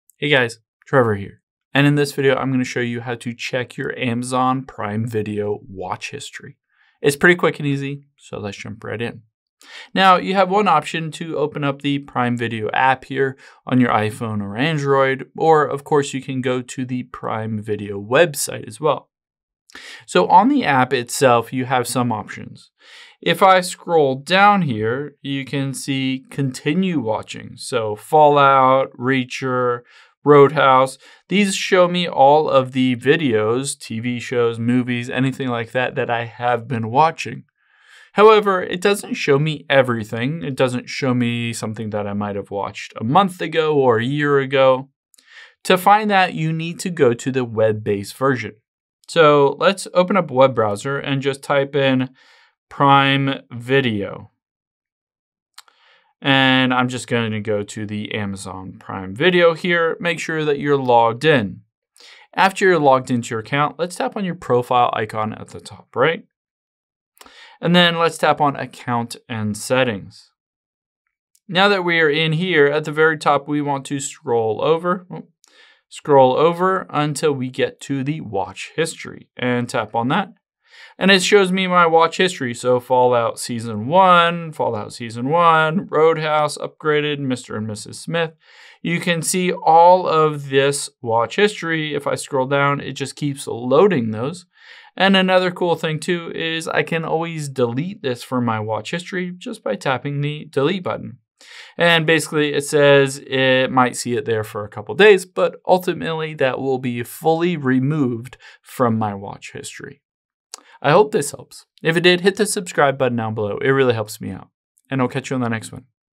How To Check Amazon Prime Video Watch History
Here's how to find your entire watch history on Amazon Prime Video to go back to shows or movies you watched in the past and delete the history if you want.